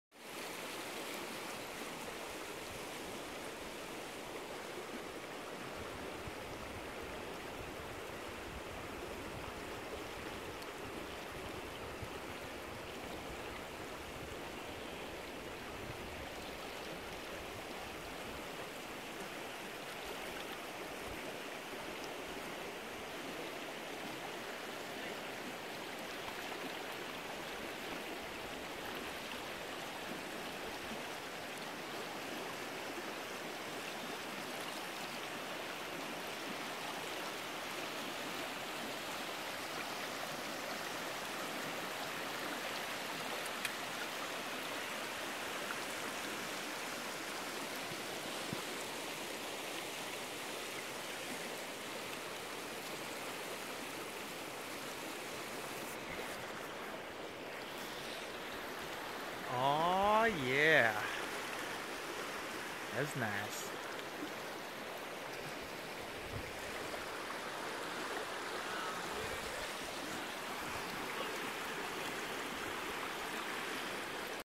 On the way up to Keyhole hotsprings.
Stopped on the side of the road to check out the river on the way up to Keyhole hotsprings.